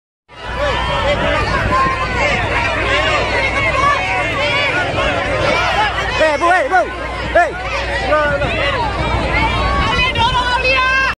proses evakuasi penduduk dan dan wisatawan yang berada di Gili Trawangan Lombok.
Senin, 6 Agustus | BeritaNasional proses evakuasi penduduk dan dan wisatawan yang berada di Gili Trawangan Lombok.